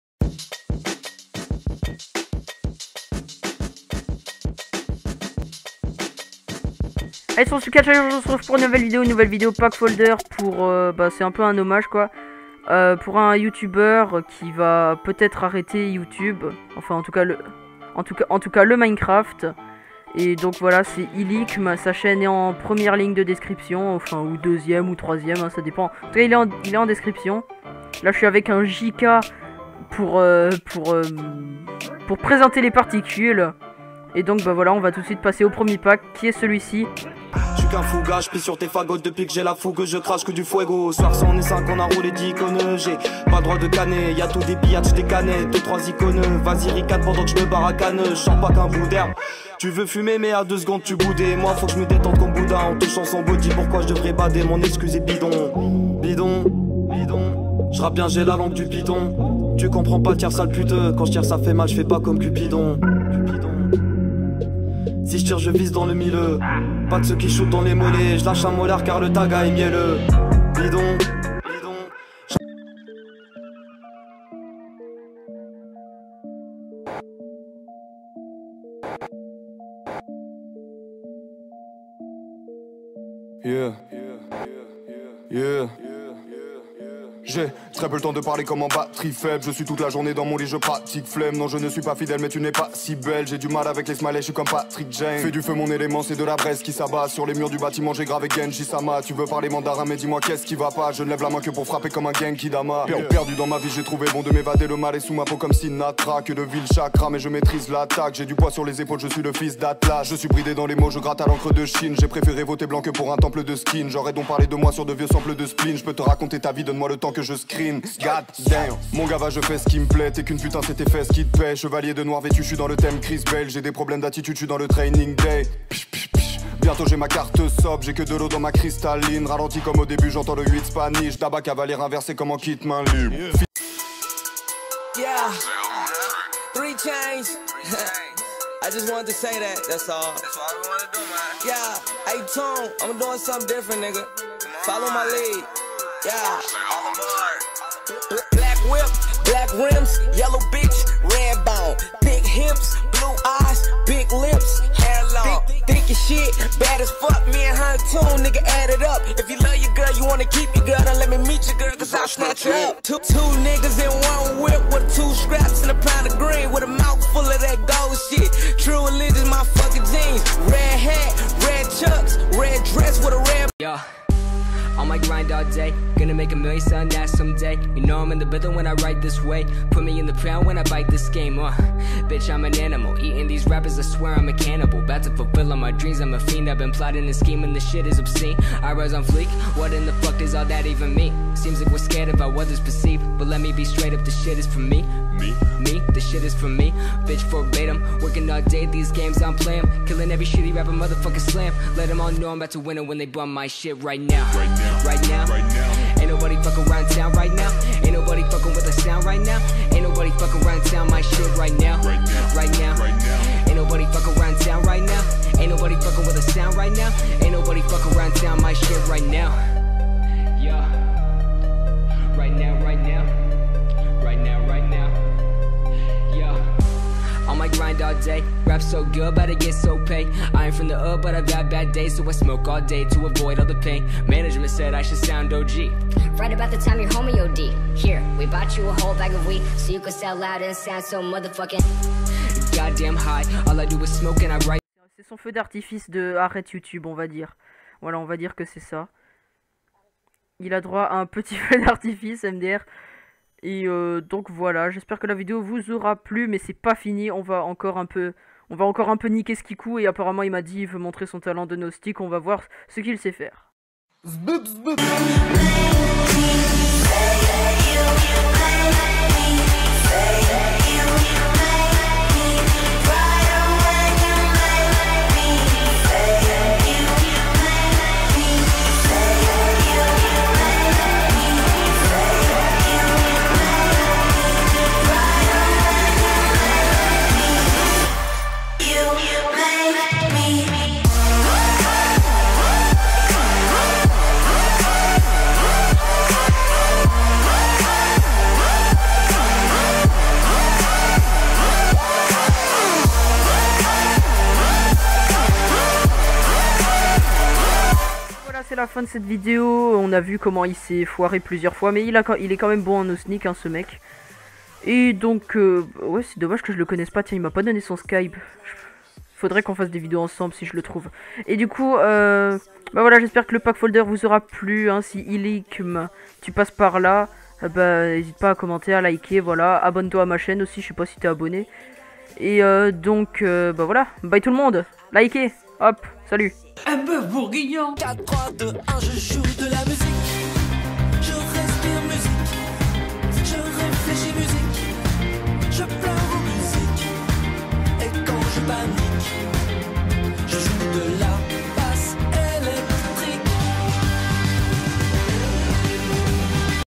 ❤️ PACK FOLDER POUR iLiqm ❤️ 😢 [2] + NO SNEAK D'UN SUB !!
Aujourd'hui vidéo pack folder "hommage" à un super youtubeur qui va malheureusement surment arreté.
50 likes pour un autre pack folder !!!